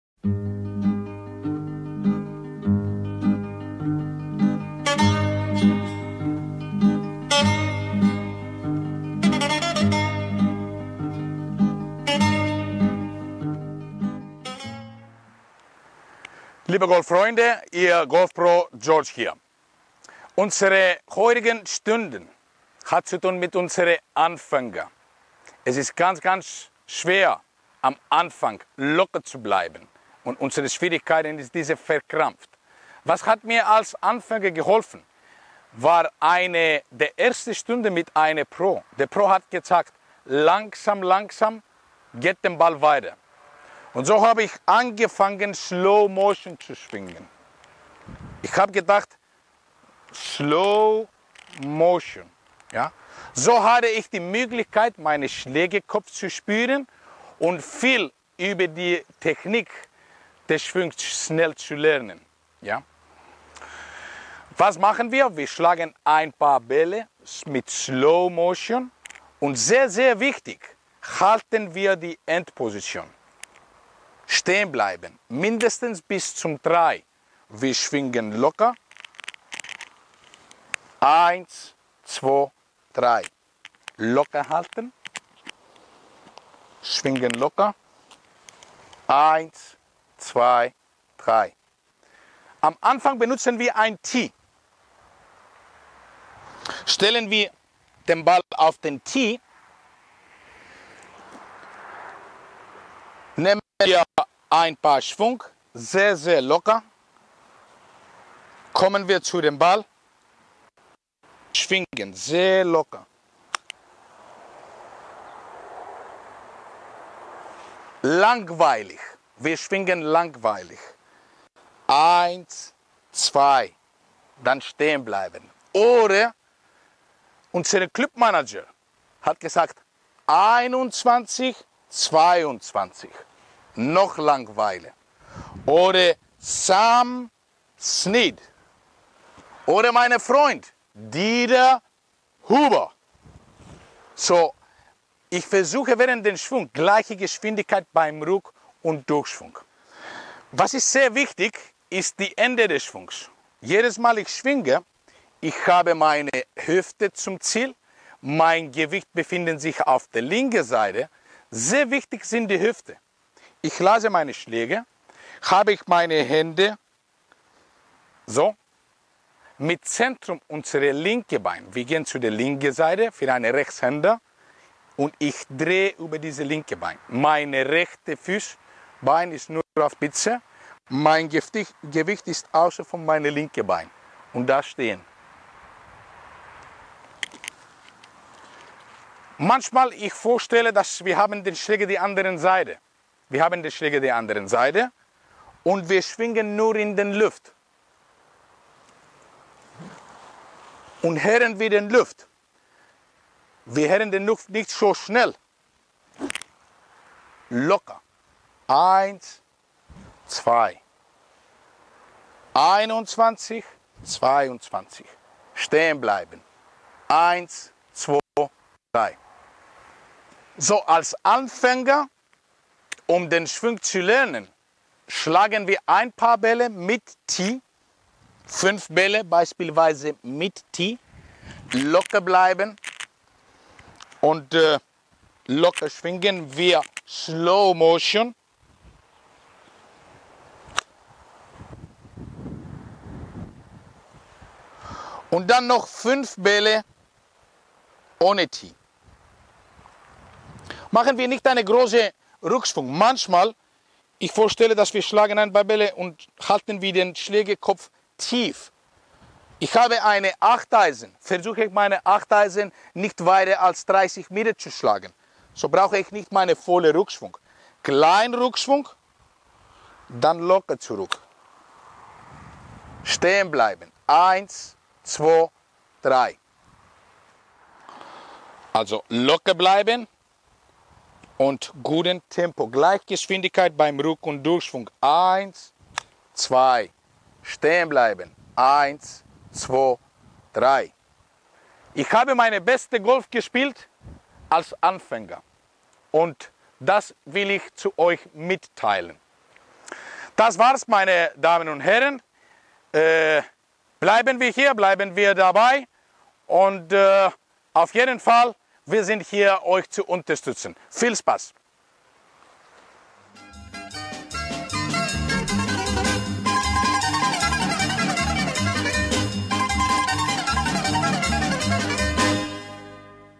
Golf Video Tipps - "Slow Motion" Schwung
Diese Golf Video Tipp ist geeignet für unsere Anfänger! Gleichzeitich alle unsere Vorgeschrittenen Spieler kann beim anschauen profitieren und ihre Spiel und ihre Rhythmus verbessern!

Tempo und Gleichgewicht zu halten während der Golf Schwung und unsere Golfrunde, 18 Löcher lang ist schwer. Hiermit werde ich mein persönliches Erlebnis mit euch austauschen und während diese Video Tipp sage ich die Wahrheit  - wie ich meine Schwung gegründet lasen habe.